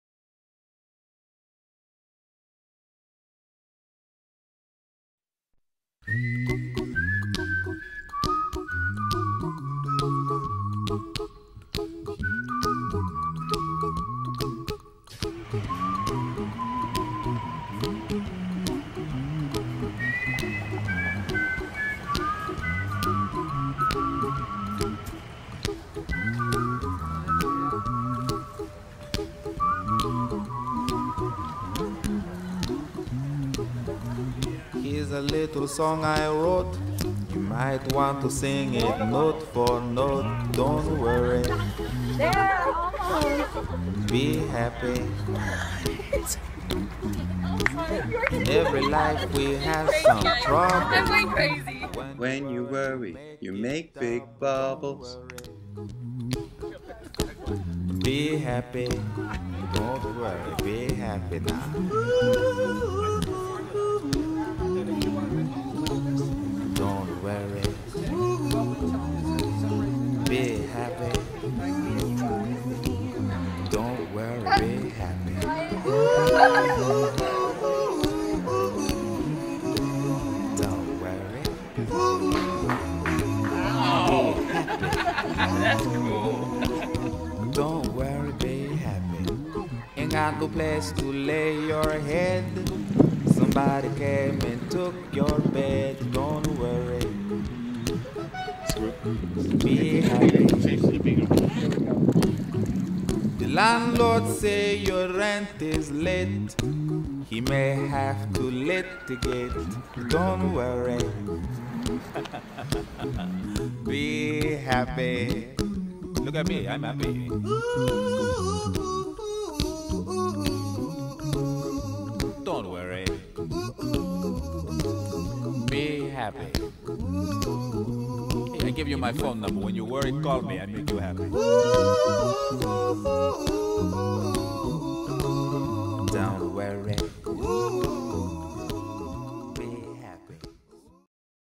Big Bubble Zone - Home of the Big Bubble Wand
Ray showing the big bubble wand in action out in the streets of Sausalito, San Francisco and Tiburon.